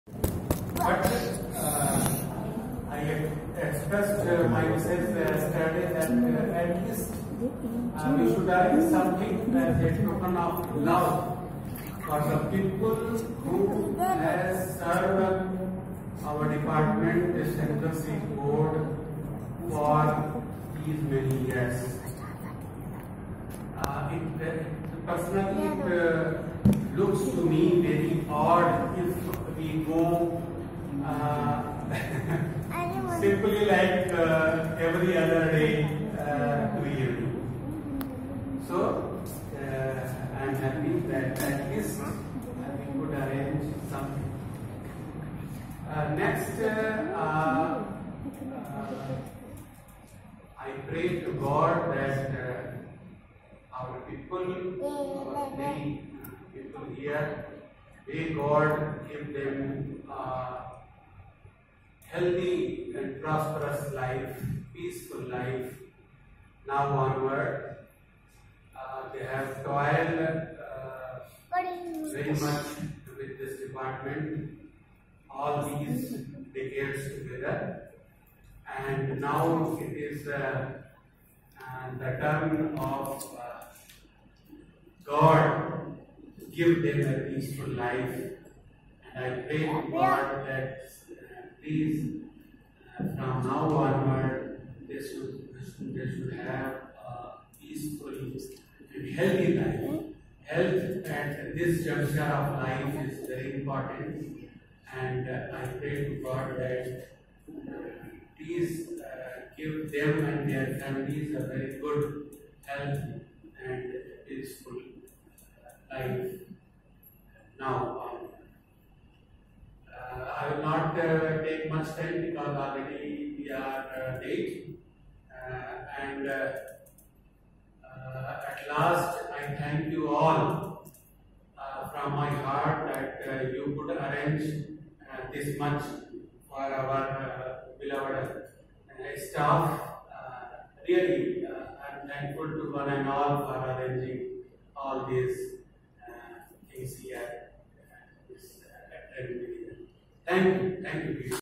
K R Ganesh Retirement
DR Pankaj Thivari speech on Ganesh retirement.